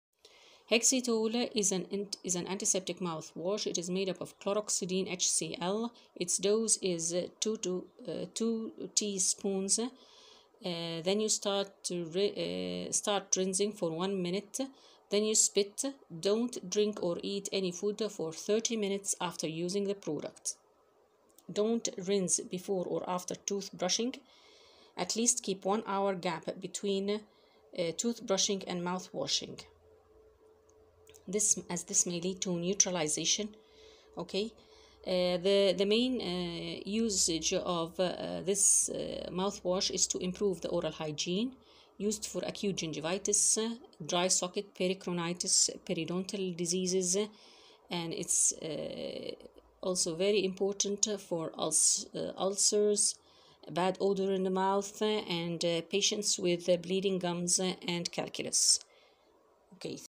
Why do we use chlorhexidine mouthwash?
we talked about the dose and uses of chlorhexidine mouthwash. Also we talked about its importance. These instructions are for getting good mouth washing results.